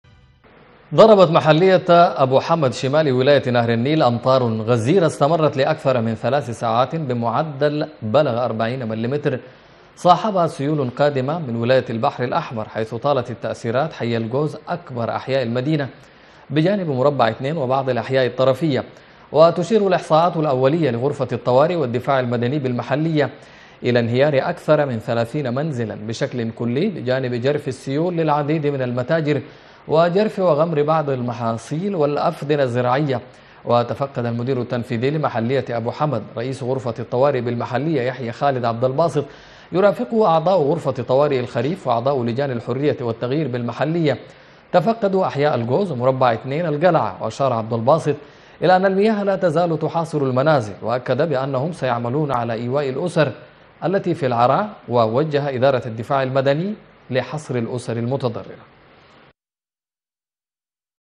أخبار | امطار وسيول تضرب محلية ابوحمد وتحدث تلفا في المباني والمحاصيل
الشروق_ الفضائية
Ashorooq Tv
ـــــــــــــــــــــــــــــــــــــــــــــــــــــــــــــ